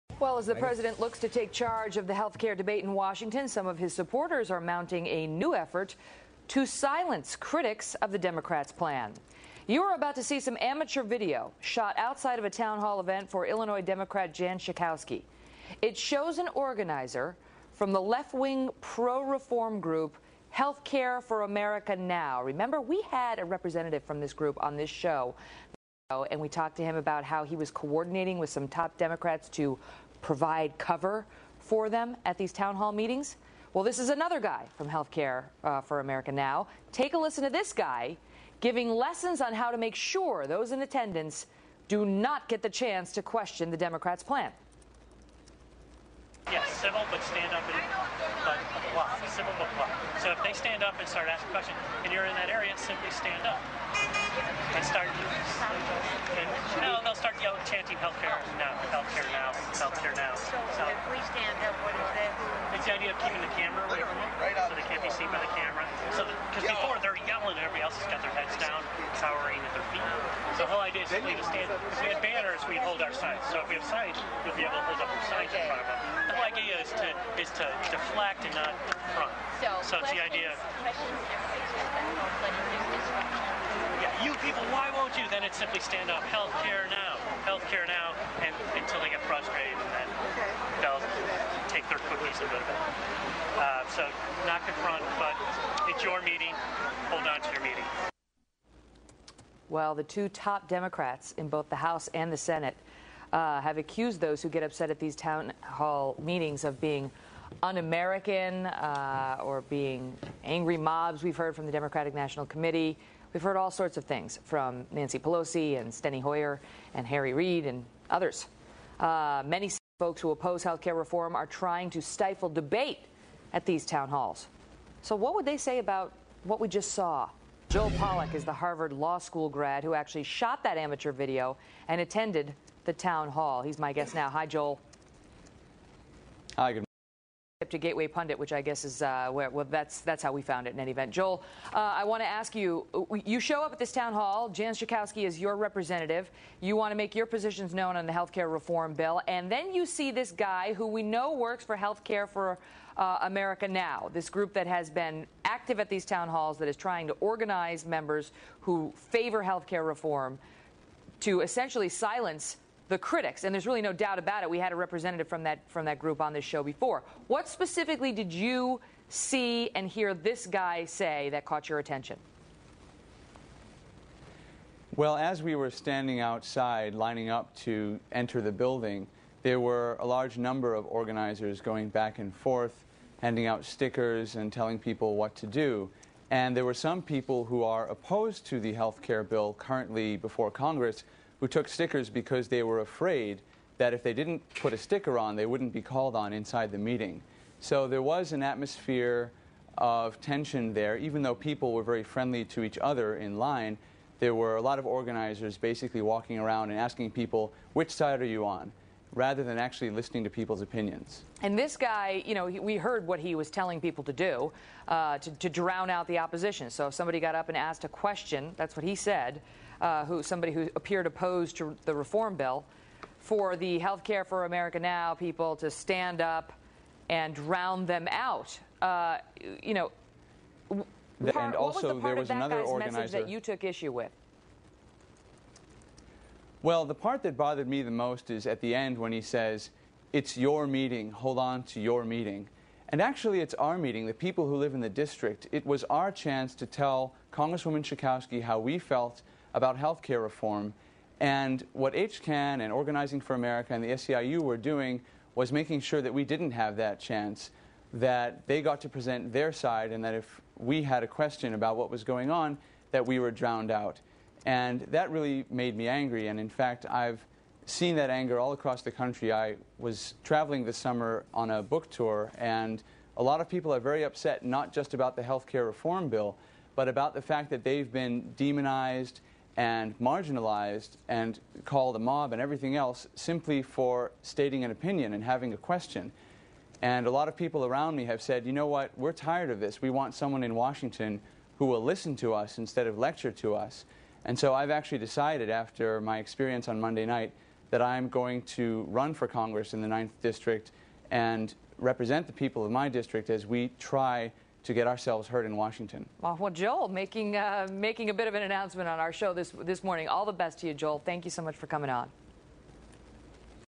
HCAN Organizer Teaches How To Disrupt Town Hall Meetings
A conservative videos a leftist HCAN organizer teaching supporters how
to stifle debate at Town Hall meetings on health care reform.